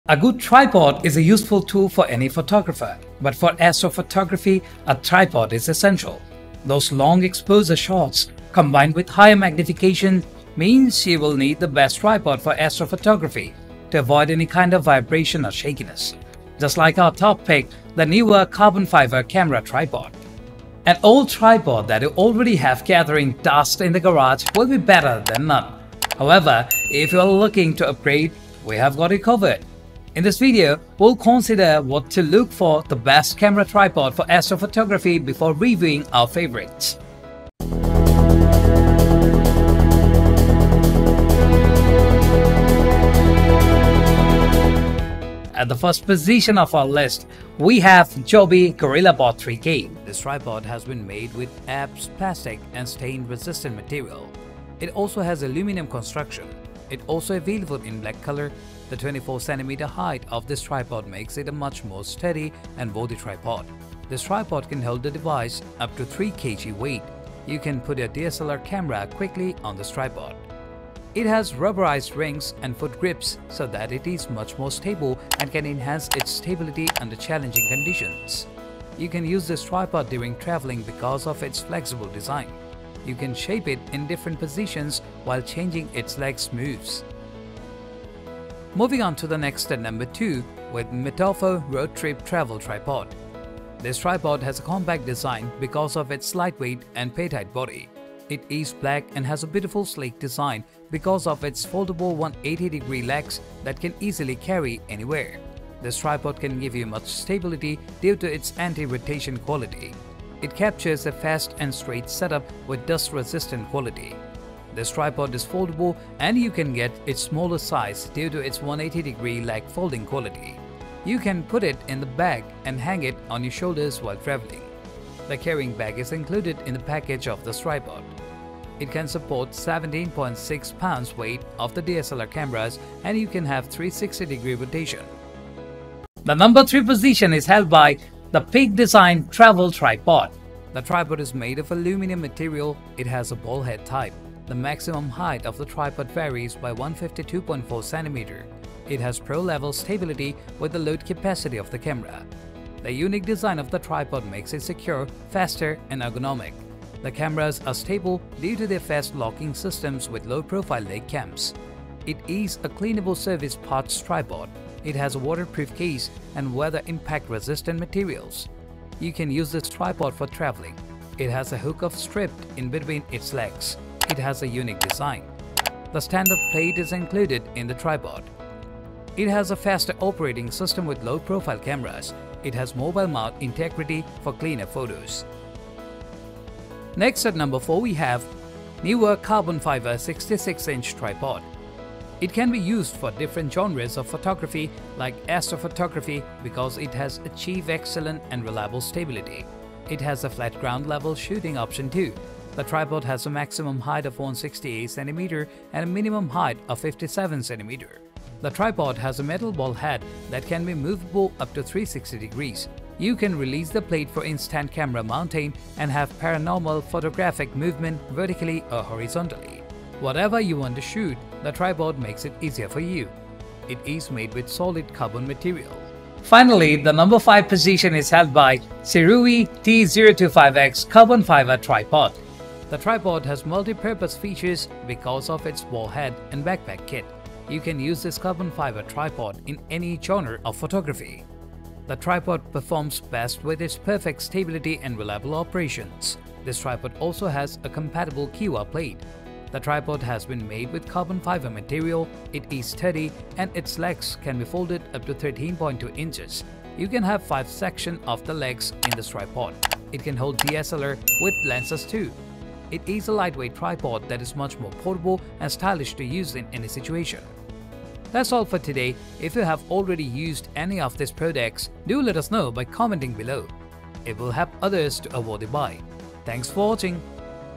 Top 5 Tripod For Astrophotography in 2024 | Detailed Reviews & Buyer's Guide
Links to the Best Tripod For Astrophotography we listed for Any Budget:

1 . Joby Gorillapod 3K

2 . MeFOTO RoadTrip Travel Tripod

3 . The Peak Design Travel Tripod

4 . Neewer Carbon Fiber 66-inch Tripod

5 . Sirui T-025x Carbon Fiber Tripod

What are the Best Tripod For Astrophotography in 2024?

In today's video we reviewed the Top 10 best Tripod For Astrophotography on the market in 2024. We made this list based on our personal opinion and we ranked them in no particular order, after doing our research based on their prices, quality, durability, brand reputation and many more.

After watching this video you will know which are the best budget, top selling and top rated Tripod For Astrophotography on the market today.

If you choose from this list you can be sure that you buy one of the best Tripod For Astrophotography available today.